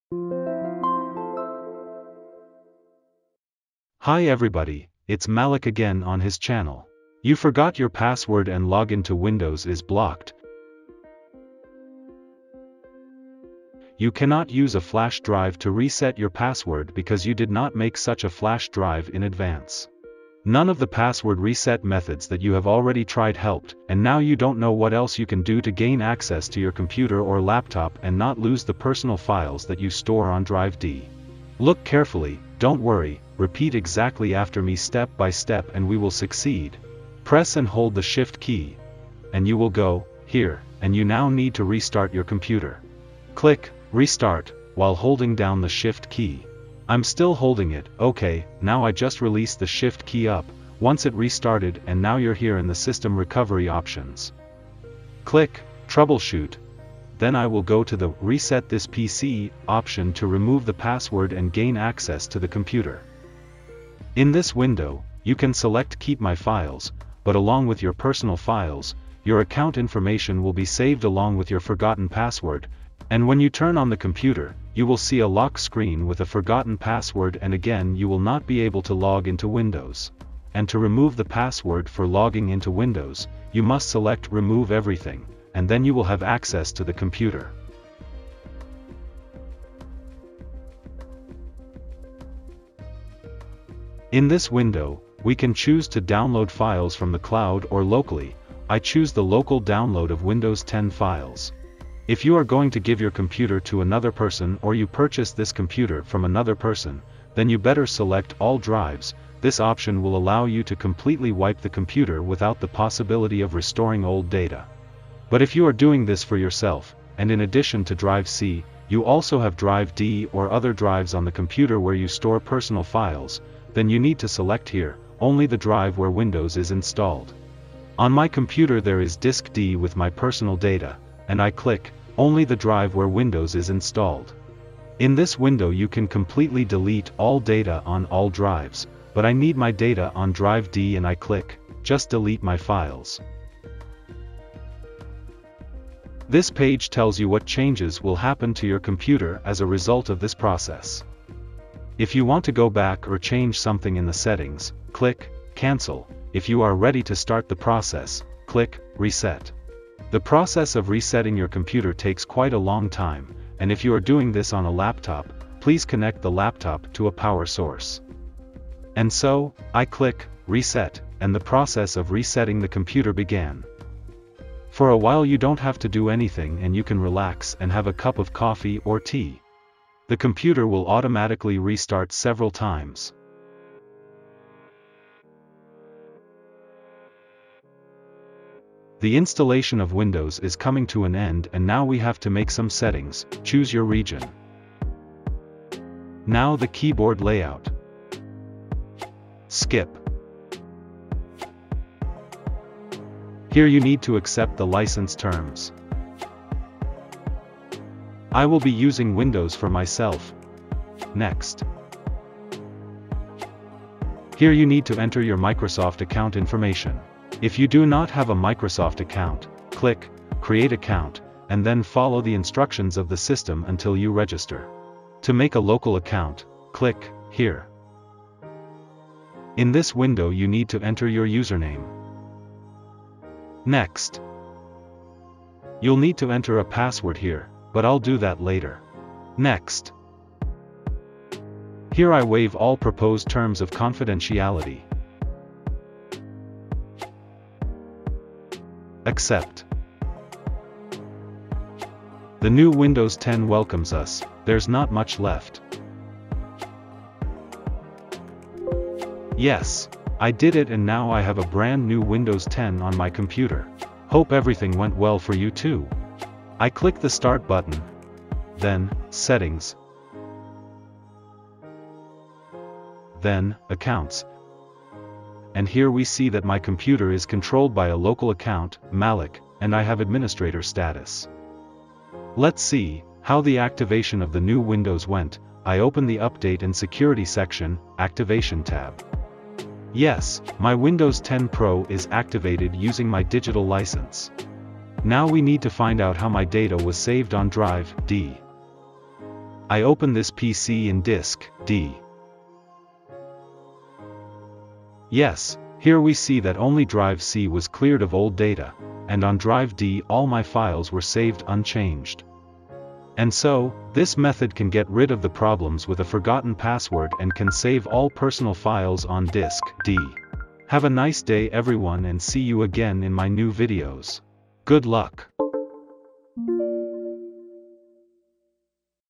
✨Forgot my PASSWORD➡️Windows is locked - I can’t login➡️Bypass Password and save data on drive D
Try PassFab 4WinKey bypass your forgotten Windows password and regain access.
✅This works in Windows 11, 10 and Windows 8.1💥
Today I will tell you what to do if you have forgotten or don’t know your Windows password and what you can do to gain access to your computer or laptop and save personal files on the D drive.
☝️This will help you if you received a locked computer from another person and do not know the password to log in.
Or you want to give your computer to someone else.